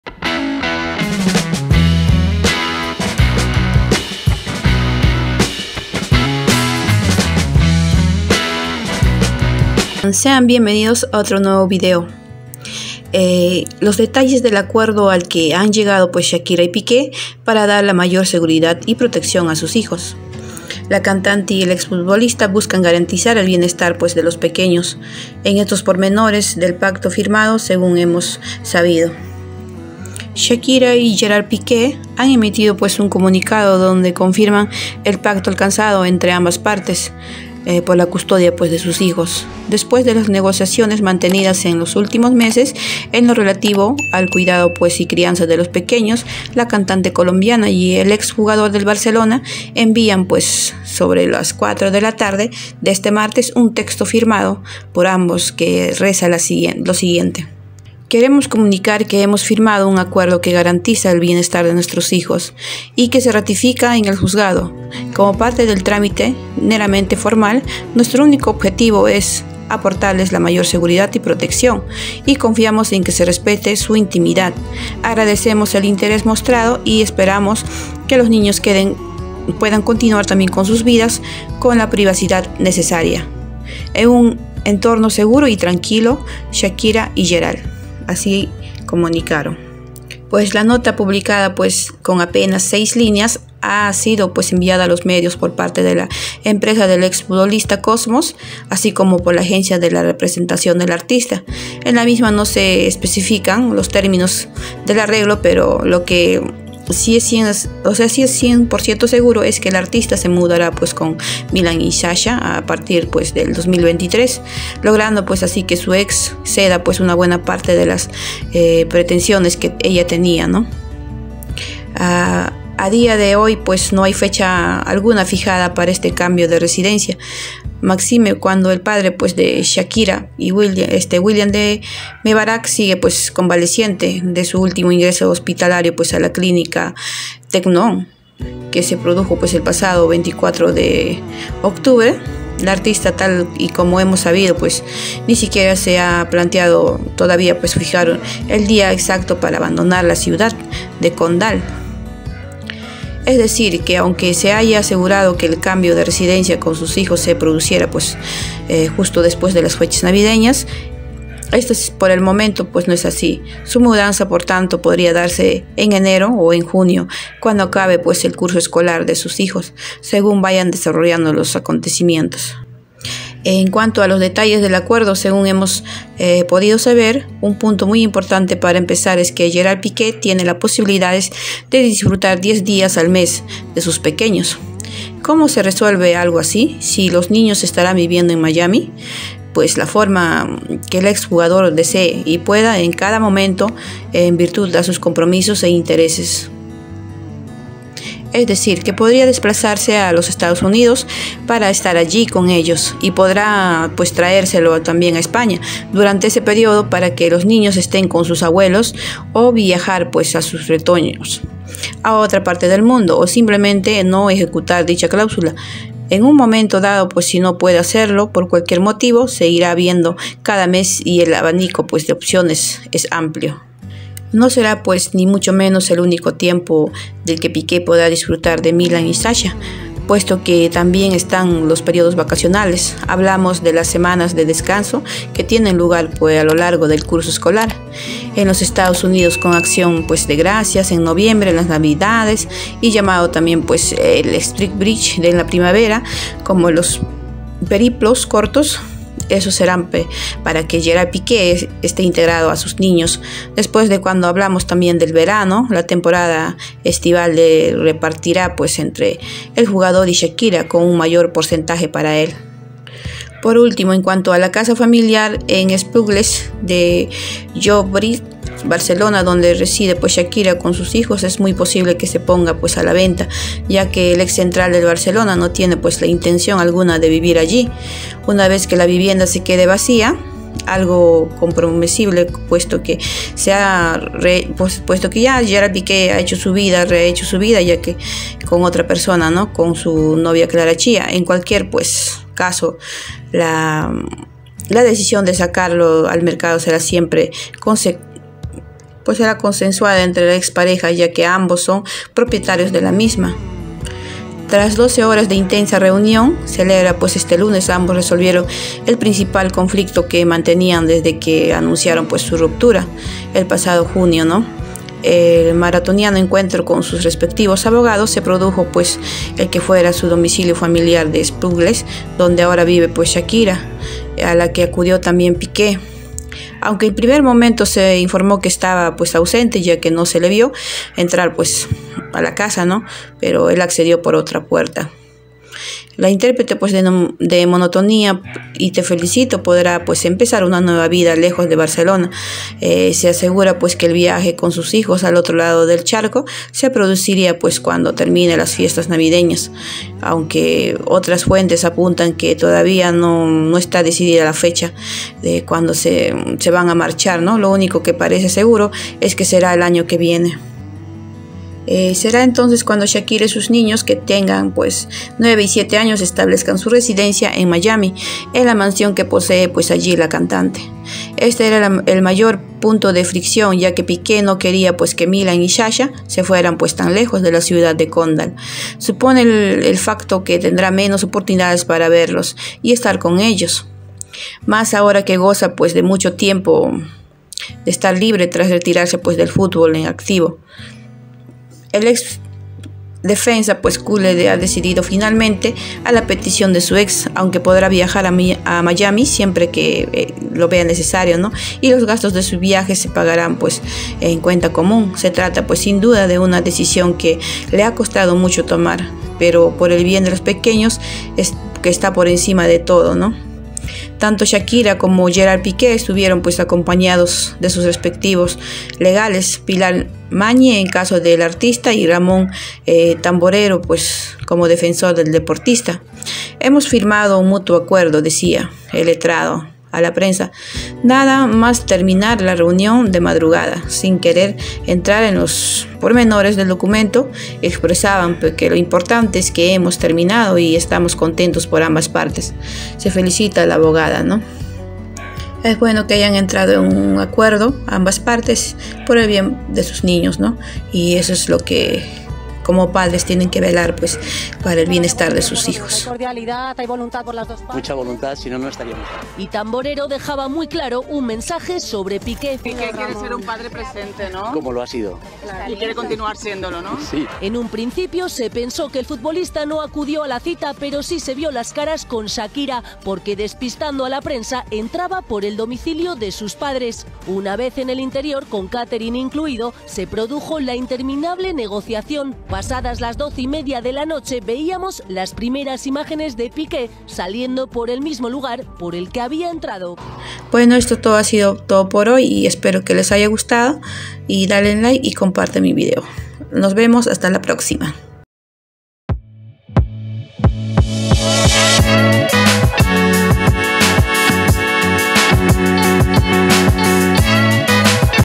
SHAKIRA Y PIQUE FIRMAN ACUERDO POR LA CUSTODIA DE SUS HIJOS
DE ULTIMA HORA SHAKIRA SE MUDA DE ESPAÑA.